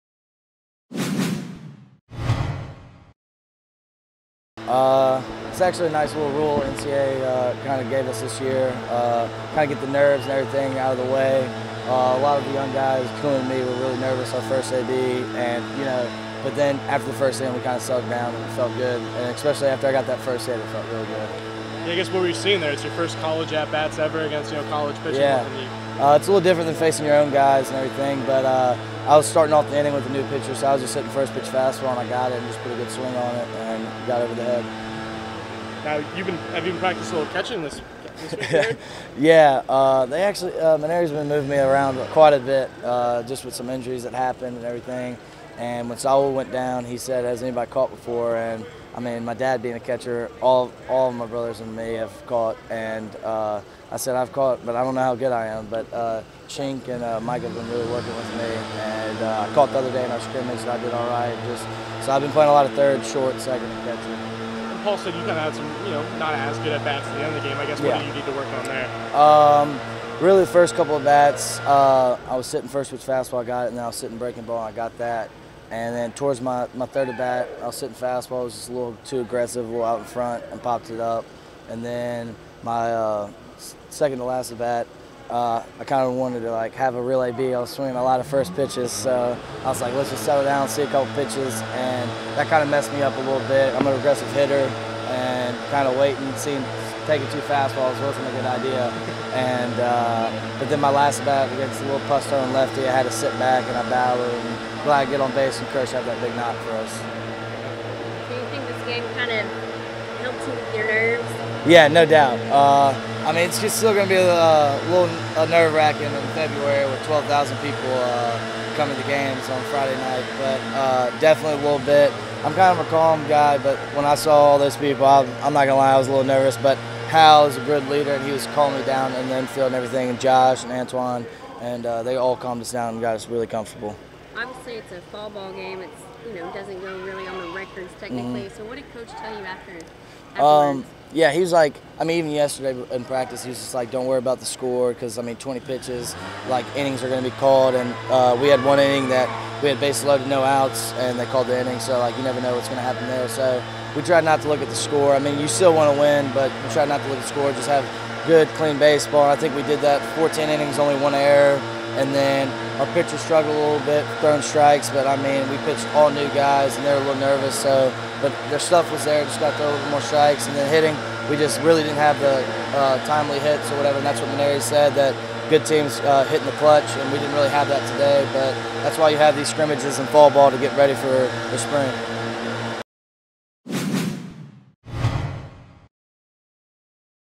"Mainieri's been moving me around quite a bit" -Freshman Drew Bianco on his versatility
Freshman Drew Bianco speaks after the Tigers 8-7 loss to the University of New Orleans Privateers in a 14-inning exhibition game.